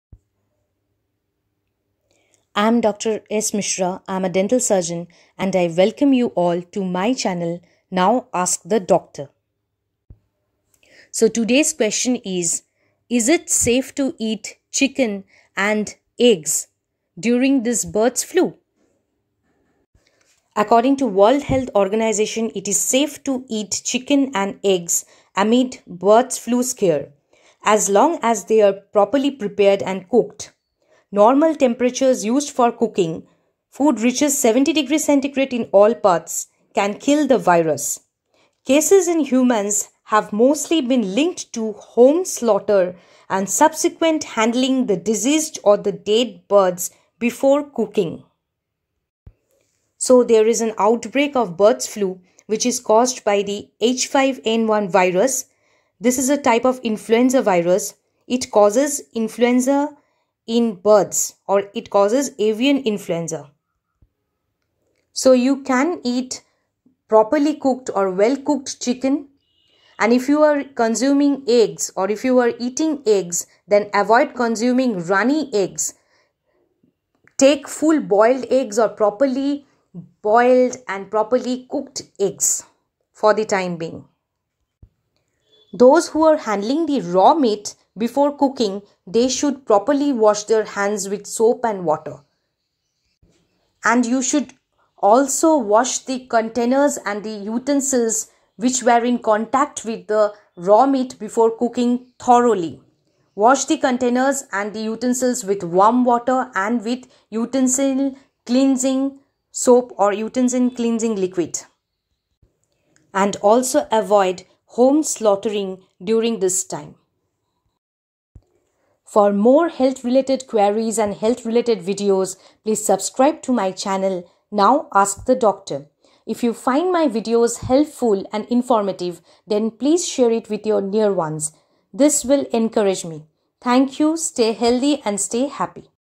Is it safe to eat eggs and chicken during Bird Flu???
All of us are nervous about Bird Flu.Bird flu is caused by H5N1 virus.It causes Avian Influenza.In this video we have explained what is safe and what is unsafe during this Bird Flu. Hope this video will help you clearing your doubt.